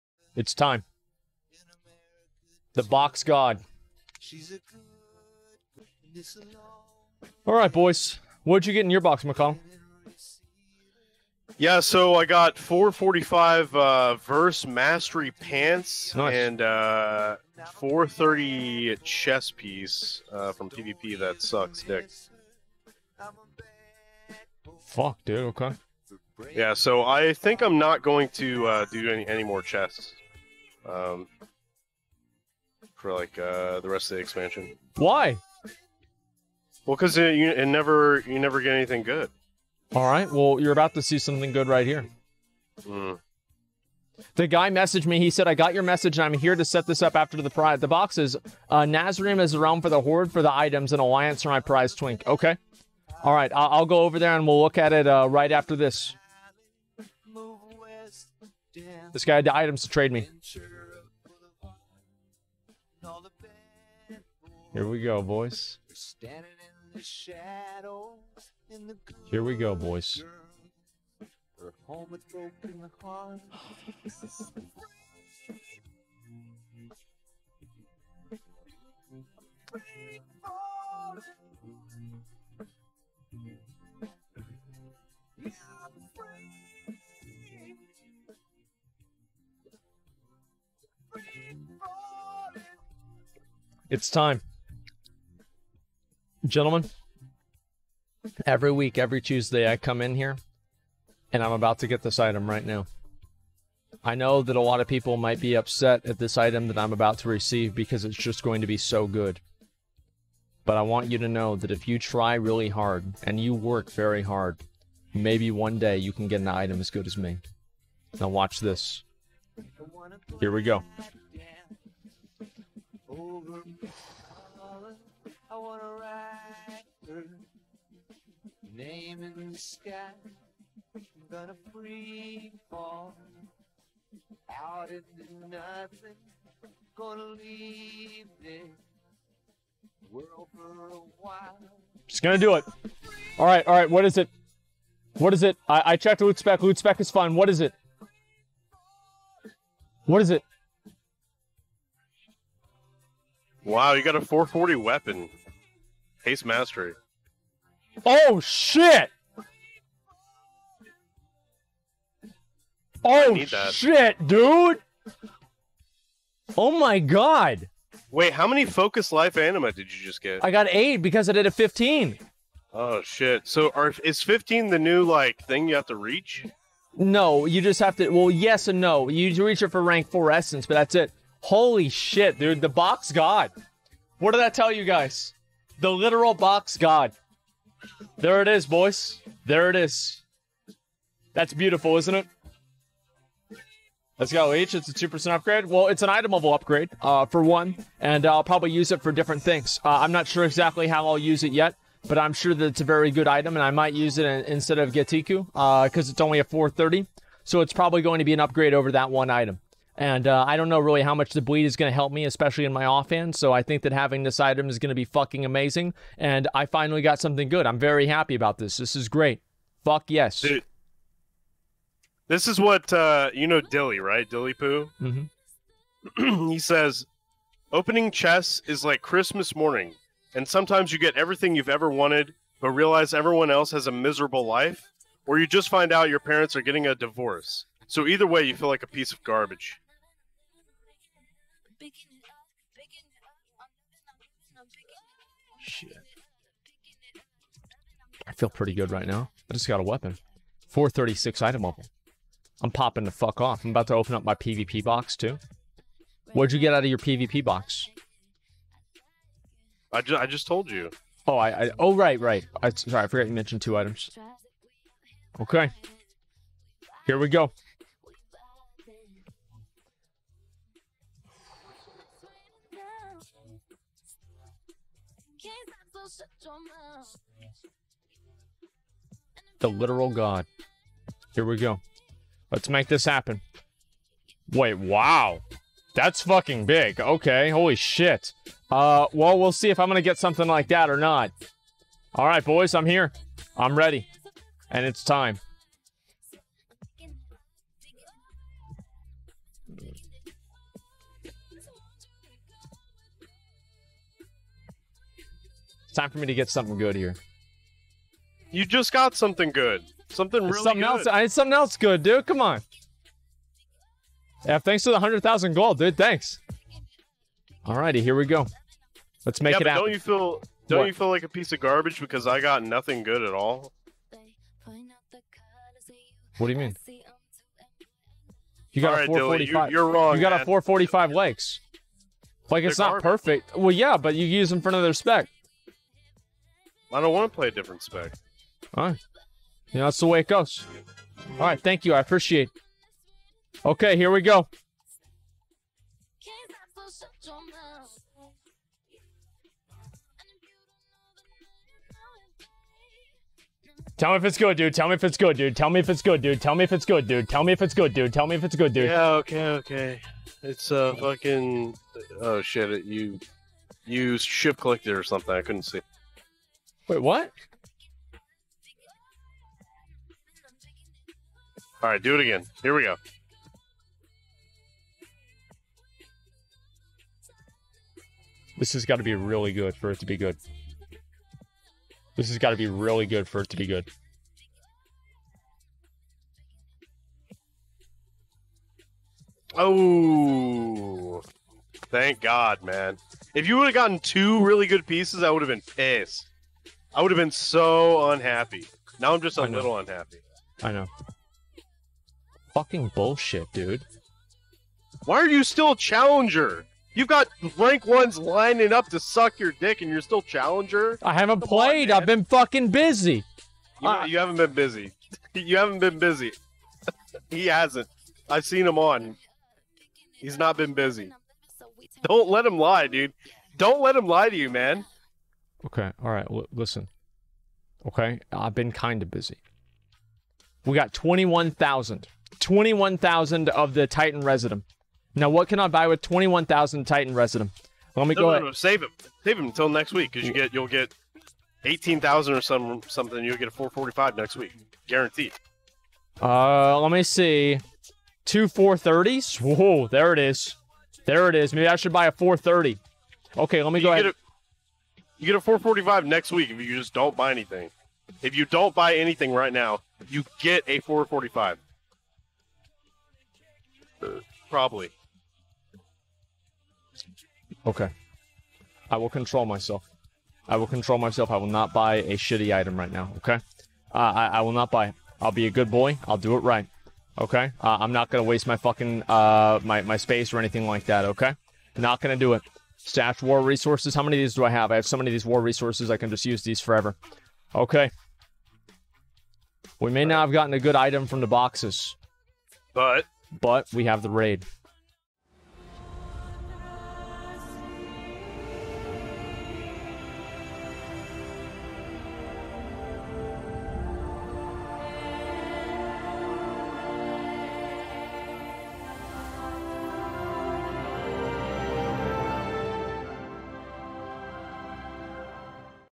Asmongold Finally Gets Something Good From His Weekly Chest (Mythic + PvP Boxes) #2
Asmongold opens his weekly Mythic and PvP boxes, finally getting something good for his spec...